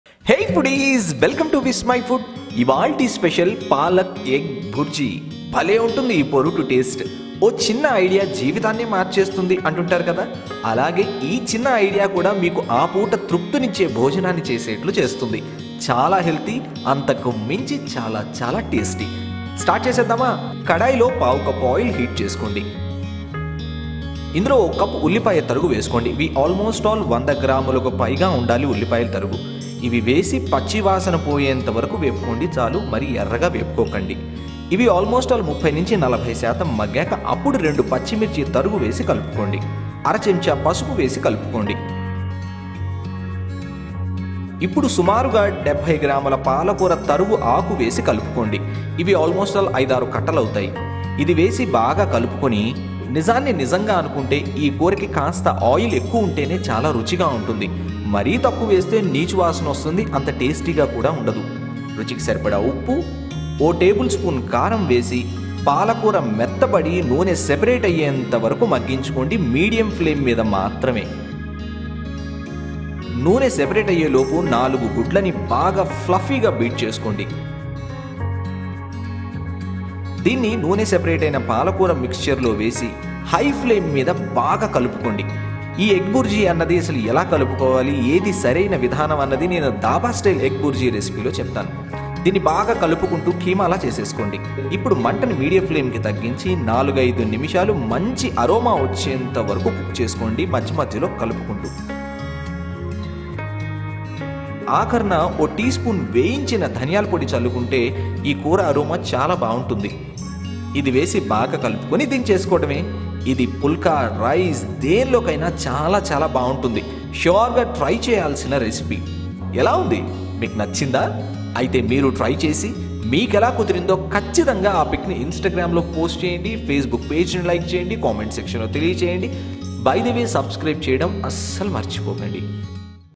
Palak Egg Bhurji | పాలకూర ఎగ్ పోరుటు | ఎగ్ ఫ్రై | ఈ ఎగ్ ఫ్రై ఆరోగ్యానికి ఆరోగ్యం రుచికి రుచి
Palak and Egg is a very healthy combination as well as tasty recipe. This healthy combination attracts even kids who hate green leaf recipes. Try out this Palak Egg Bhurji at your home and share the best with your dear ones.

Ingredients

1/4 Cup Oil
4 Eggs
100 Grams Onions Chopped
2 Green Chillies
1/4 Table Spoons Turmeric
70 Grams Palak Chopped (5-6 Bunches)
Salt
1 Table Spoon Chilly Powder
1 Table Spoon Coriander Powder